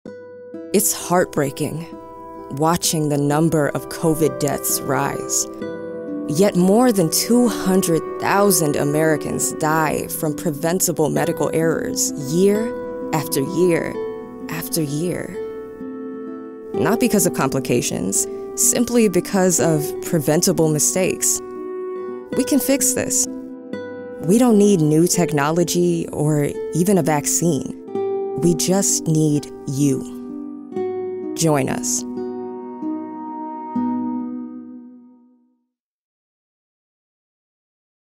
What kills more people than COVID-19 every single year? Medical Errors.
More than 200,000 die from preventable errors every year, we can fix this. We just need you!